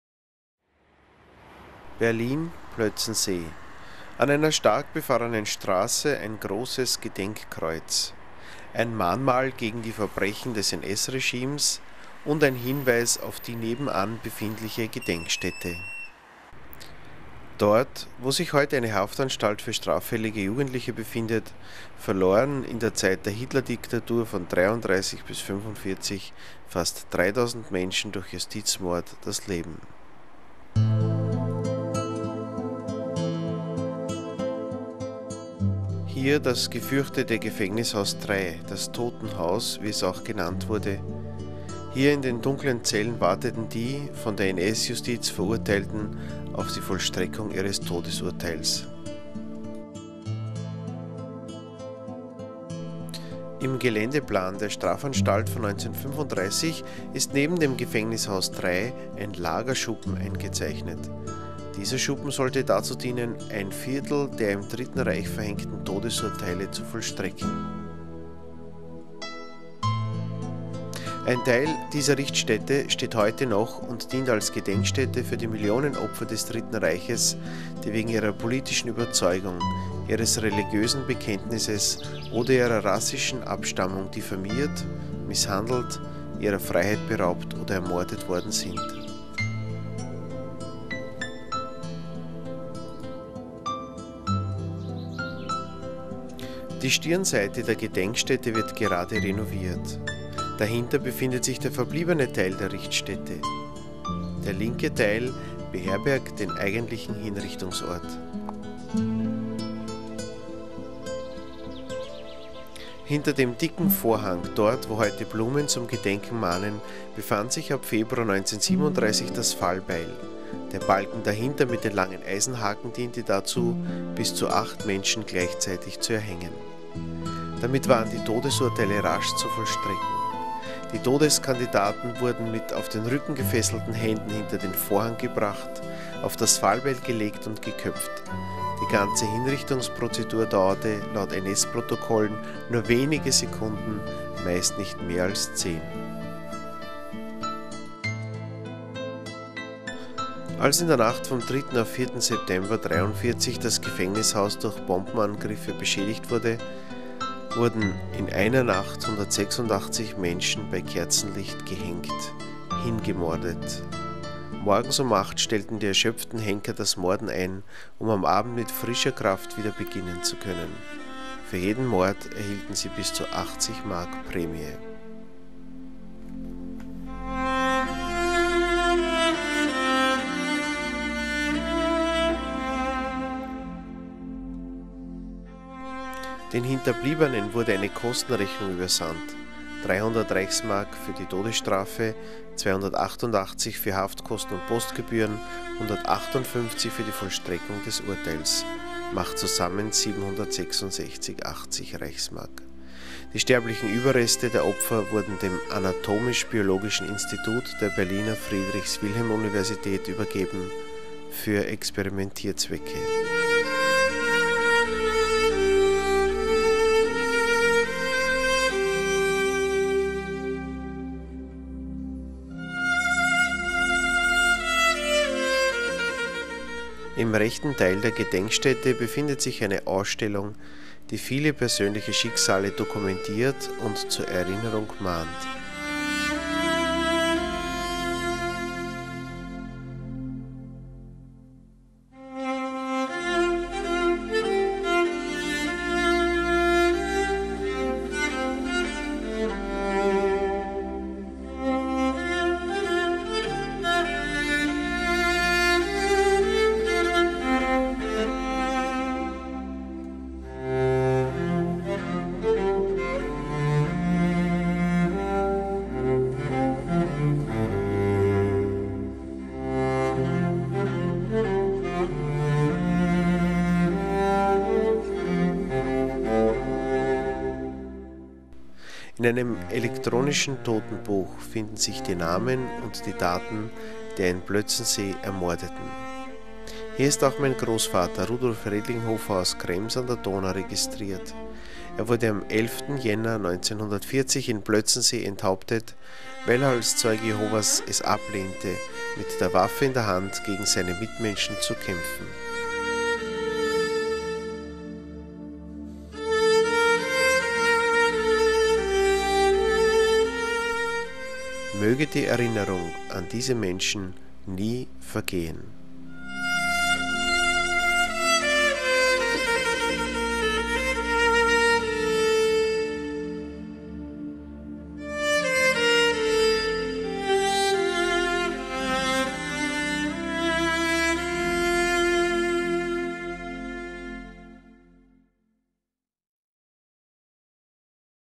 Rudolf Redlinghofer - hingerichtet in Berlin-Plötzensee
Dieses Video beschreibt anhand des NS-Opfers Rudolf Redlinghofer aus Krems (Österreich) die Leidensgeschichte vieler unschuldiger Opfer des NS-Regimes.